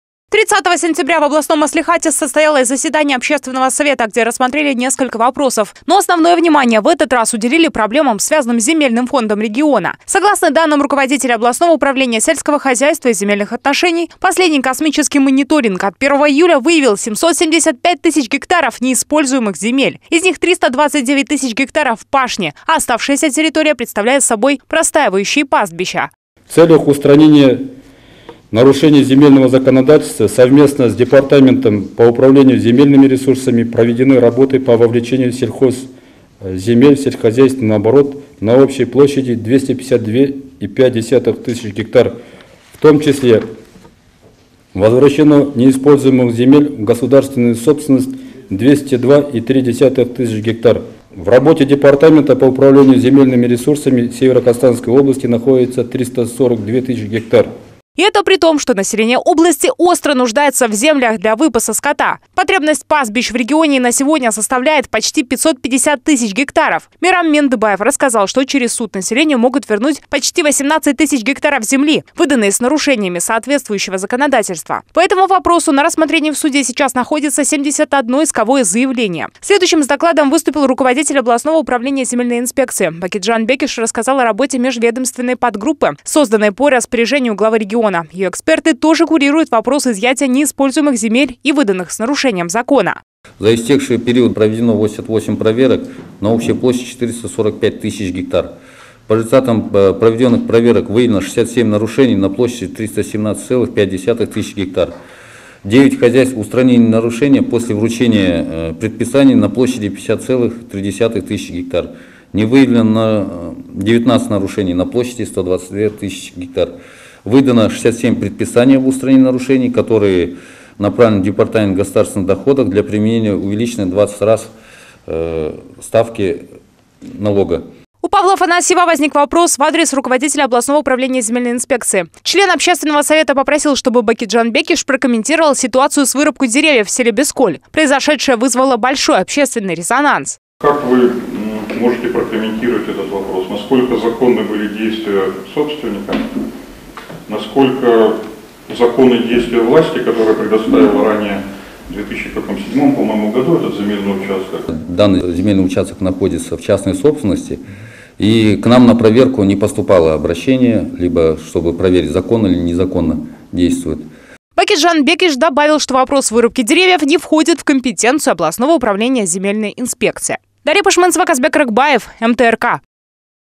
На заседании областного общественного совета обсудили земельные вопросы
30 сентября в областном маслихате состоялось заседание общественного совета, где рассмотрели несколько вопросов. Но, основное внимание в этот раз уделили проблемам, связанным с земельным фондом региона. Согласно данным руководителя областного управления сельского хозяйства и земельных отношений, последний космический мониторинг от первого июля выявил 775 тыс га неиспользуемых земель. Из них 329 тыс га пашни, а оставшаяся территория представляет собой простаивающие пастбища.